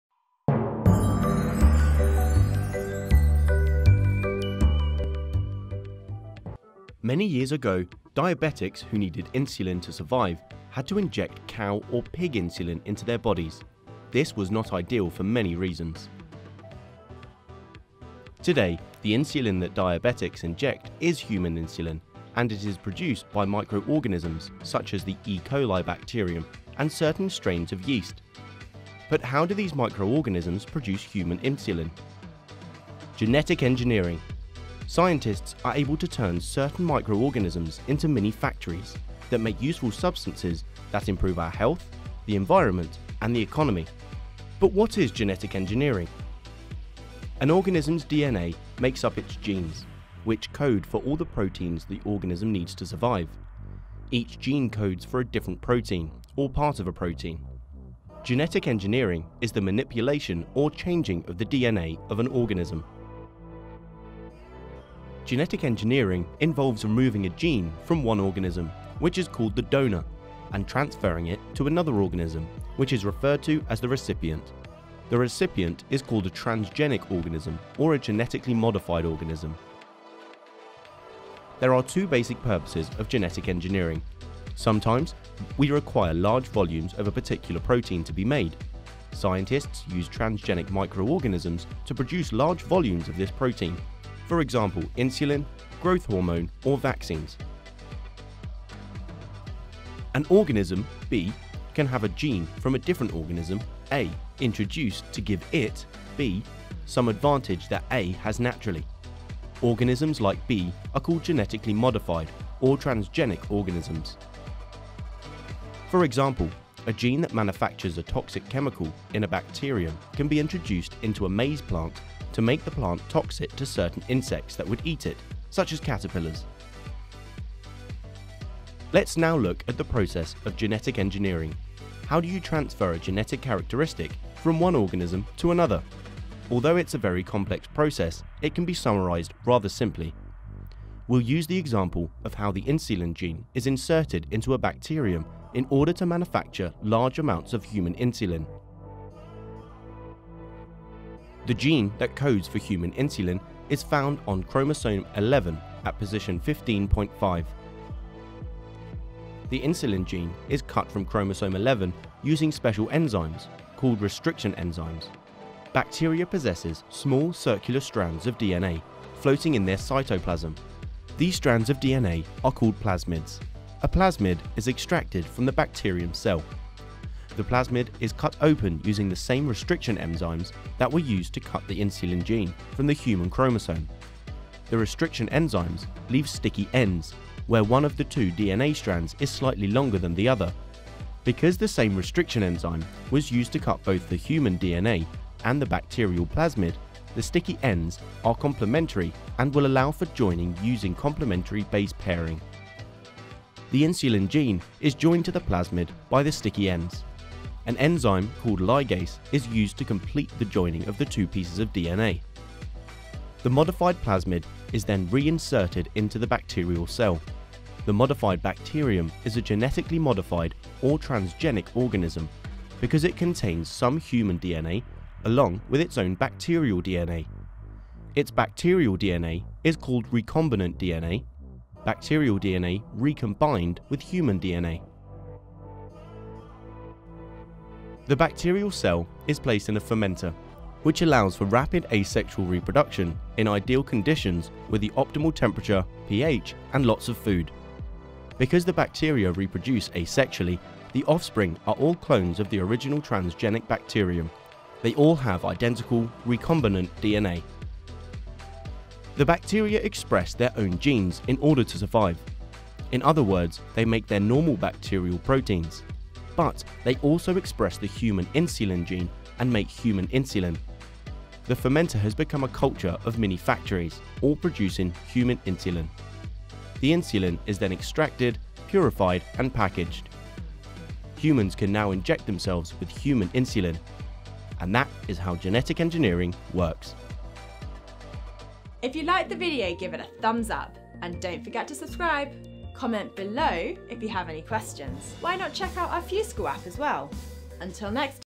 Genetic engineering | Genetics | Biology | FuseSchool
In this video we'll go in depth with genetic engineering; on how it is made and what is it used for!
Keep watching to unravel the meaning of genetics!

CREDITS
Animation & Design: Chloe Fyvie Adams
Narration: Dale Bennett
Script: Helen de Wet